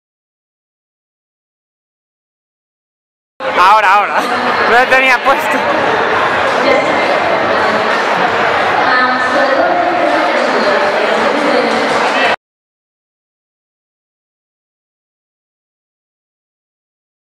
Japan Weekend Madrid 2017 - D.Va (Overwatch)
Fabuloso cosplay de D.Va de Overwatch visto en la Japan Weekend de Madrid (Febrero 2017)

Cosplayer: CheyChey Cosplay